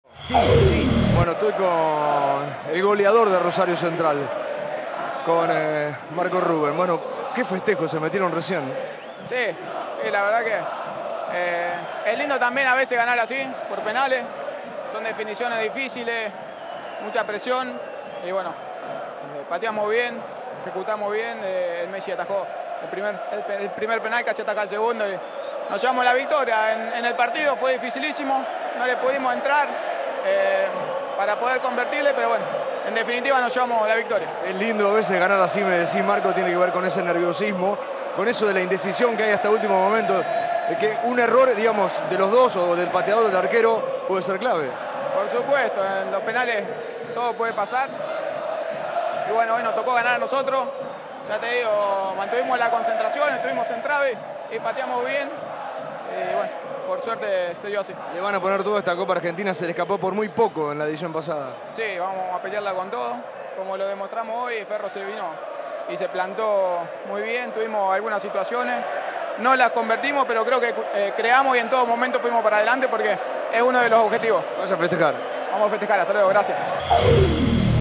Ruben:"A veces es lindo ganar así". Central 0 (5) Ferro 0 (3).Copa Argentina.FPT
Marco Ruben analizó el partido y aseguró que la Copa Argentina es "uno de los objetivos". El delantero reveló que "es lindo ganar por penales".